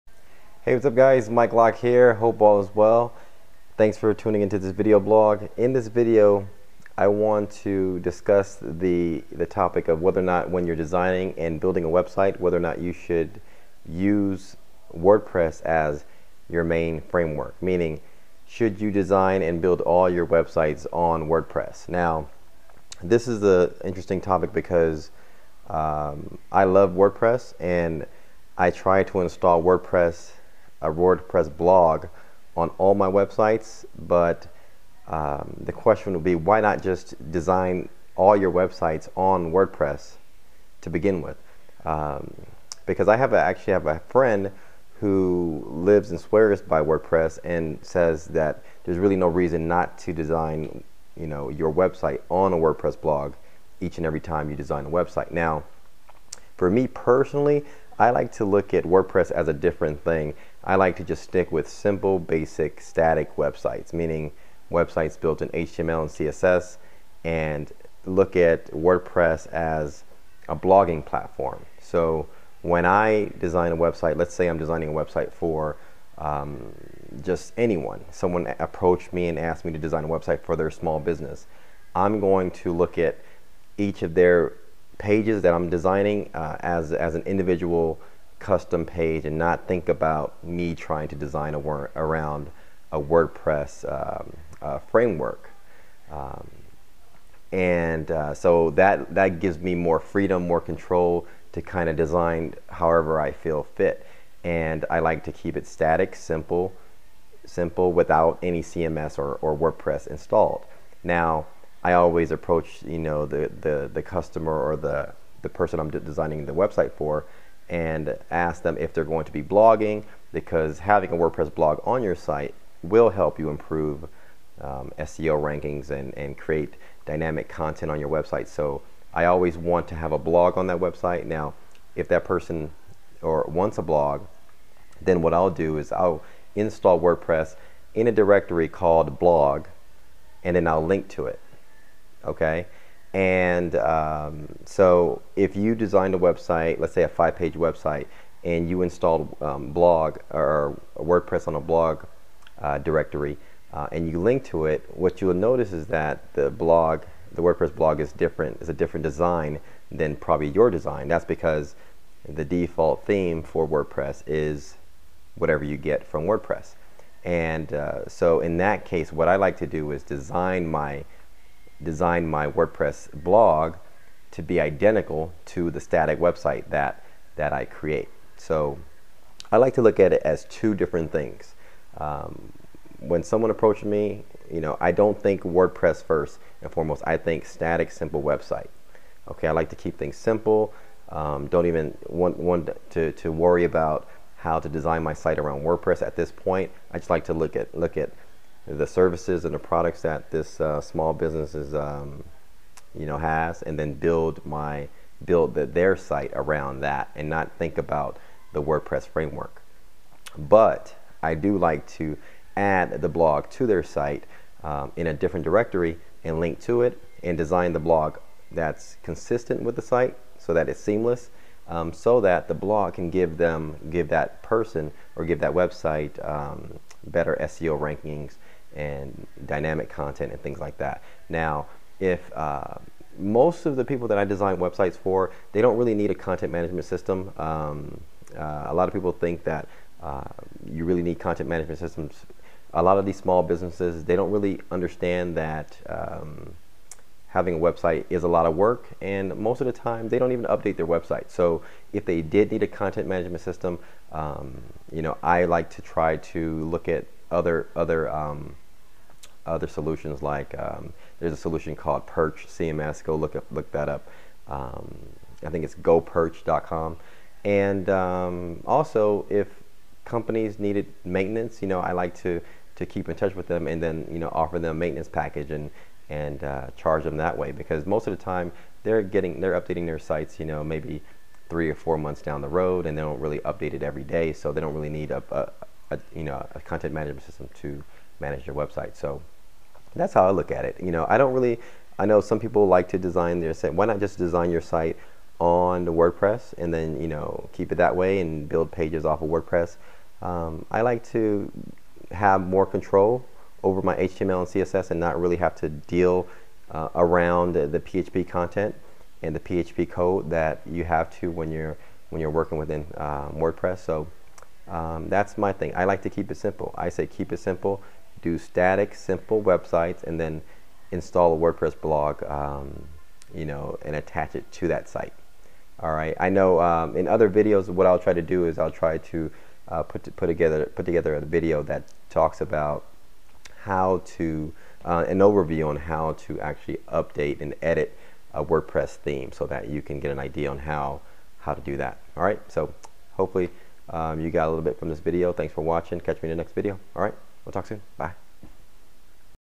Should you Build all your Websites on Wordpress?
In this video I talk about whether or not you should build all your websites on and around the Wordpress framework.